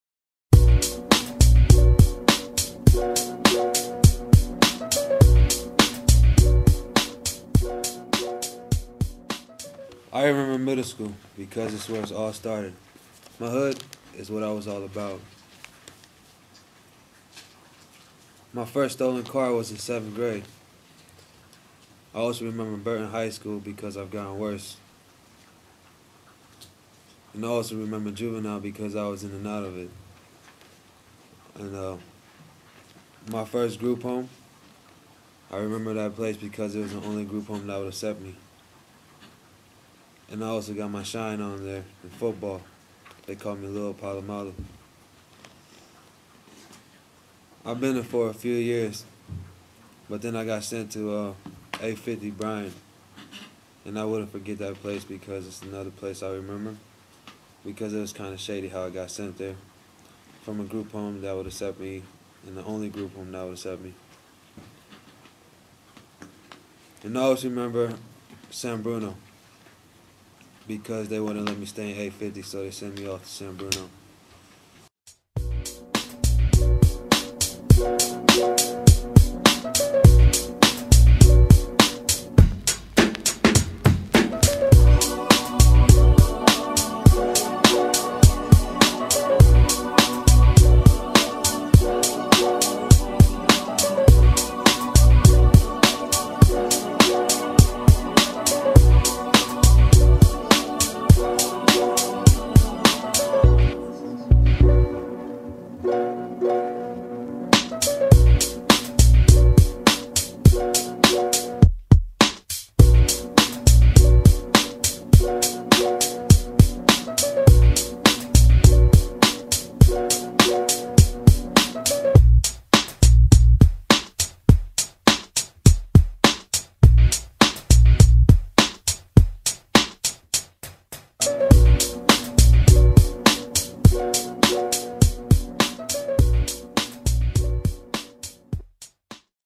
I Remember by barry
A student talks about what he remembers about his childhood, and he can still be able to smile today after everything he went through.